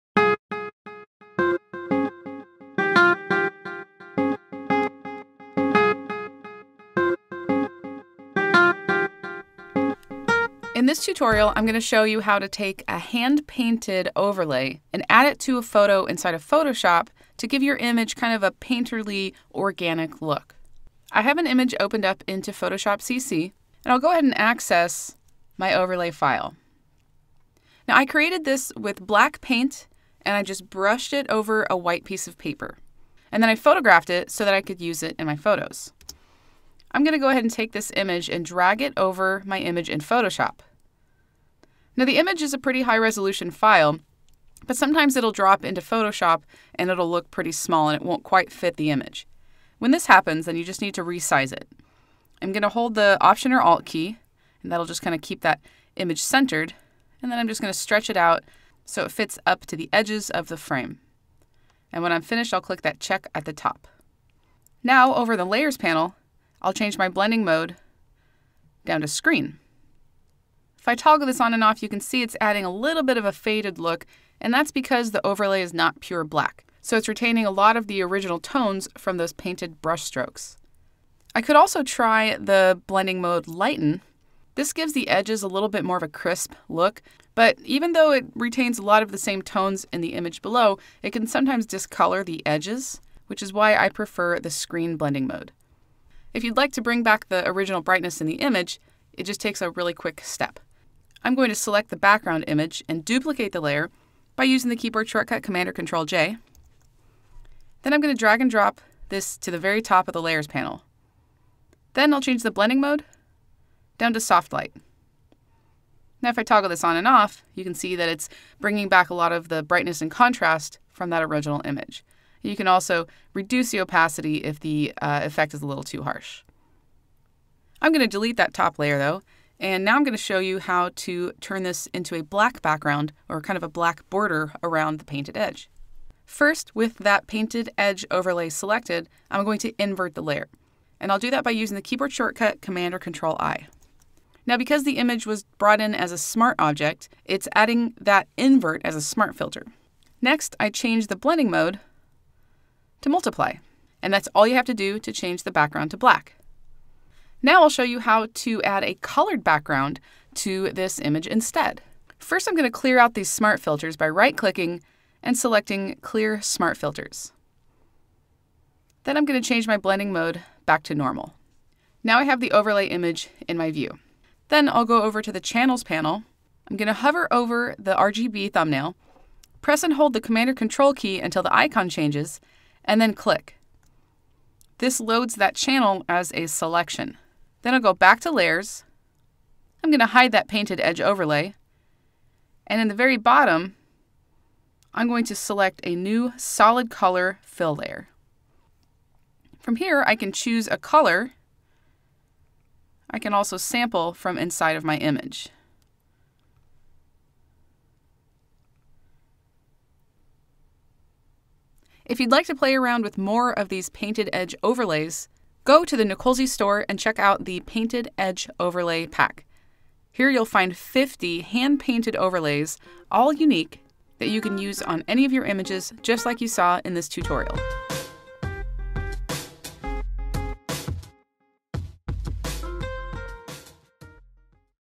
Adding a Painted Edge in Photoshop
Learn how to use a painted overlay to give your photos a painterly, organic look. Plus, download the overlay used in this video for FREE!